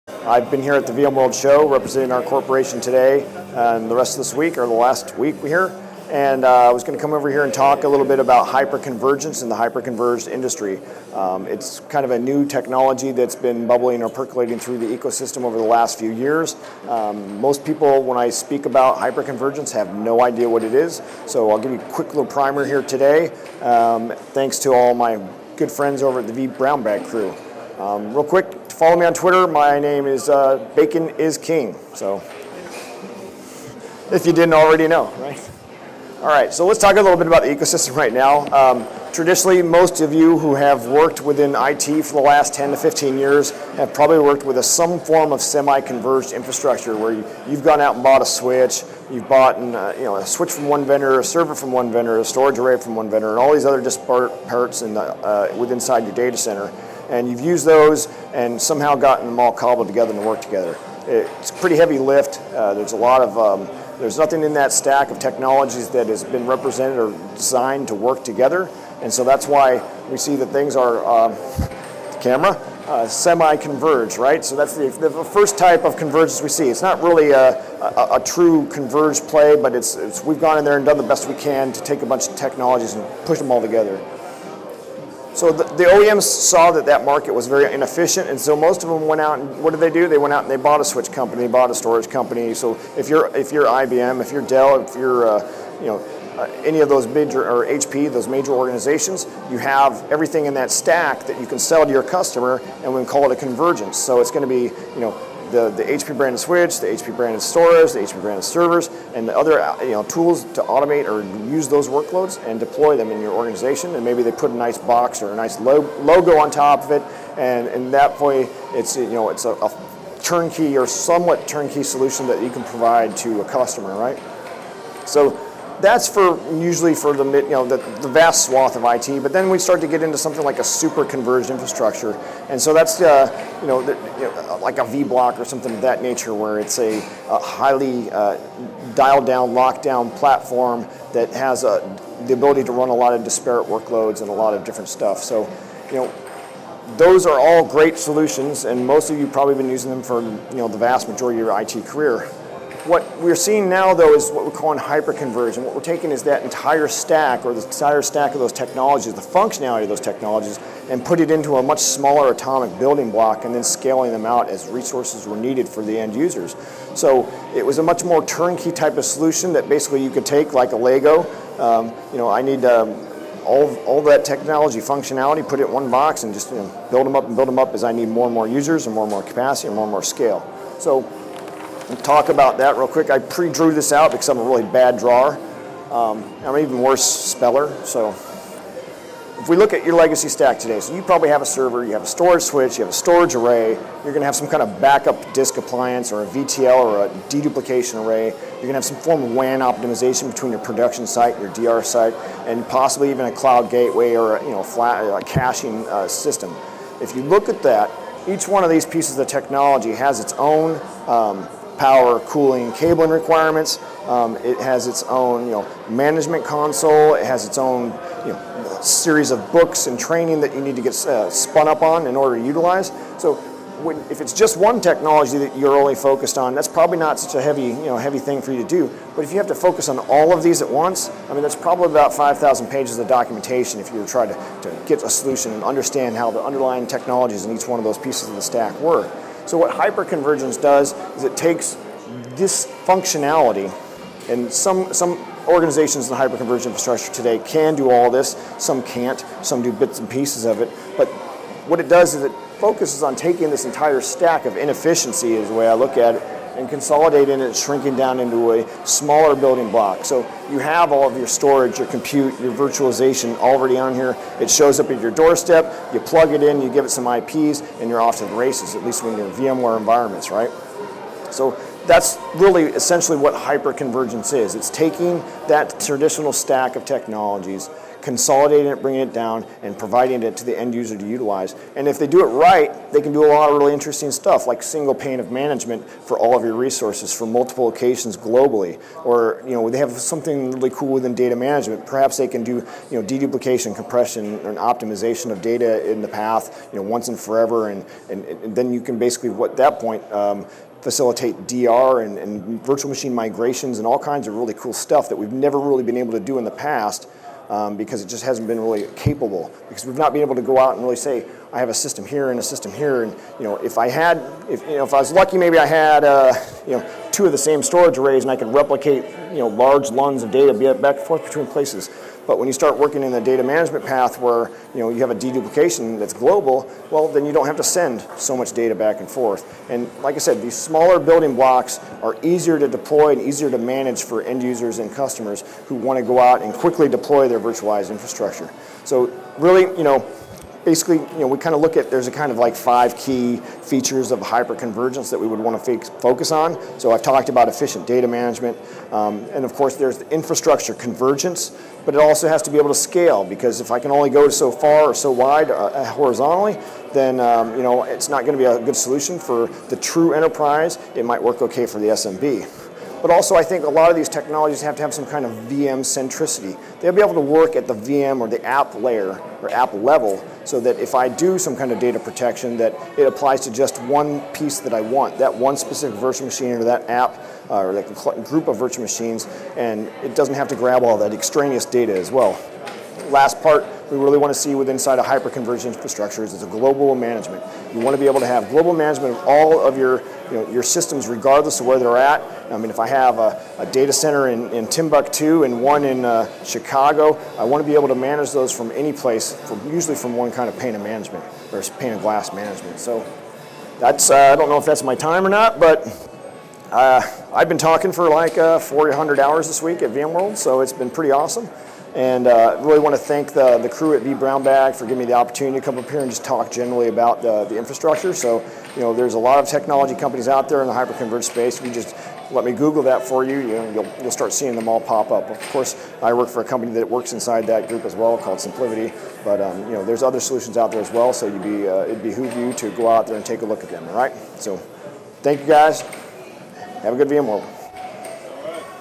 Tech Talks: Gabriel Chapman Answers the Question: What is Hyper Convergence?
Tech Talks at the The Do-It-Yourself Video Studio for VMworld U.S. 2013.